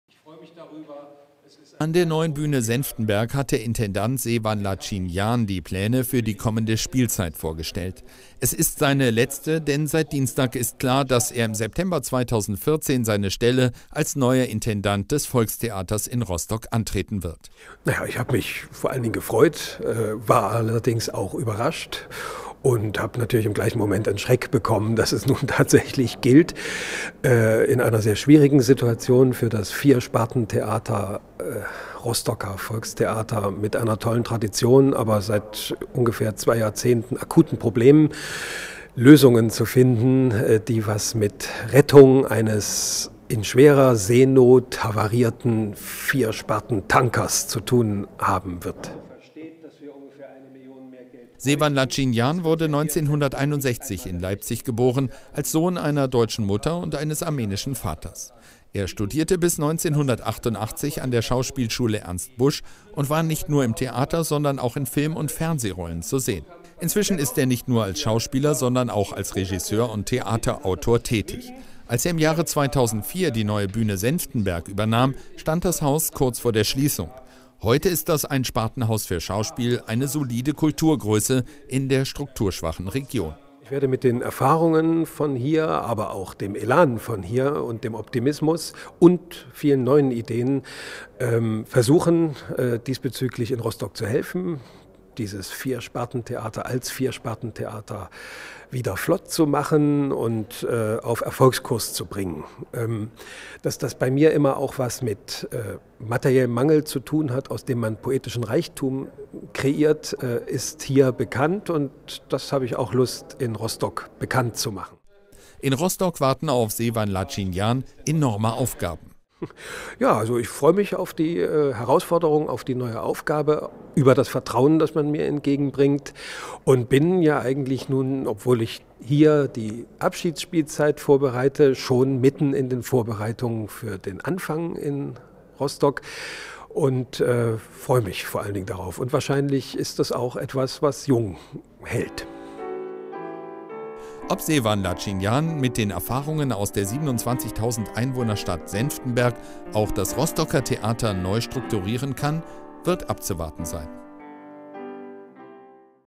Sewan Latchinian
Im brandenburgischen Senftenberg stellte der Intendant Sewan Latchinian den neuen Spielplan vor. Im September 2014 wird er Intendant des Volkstheaters Rostock sein.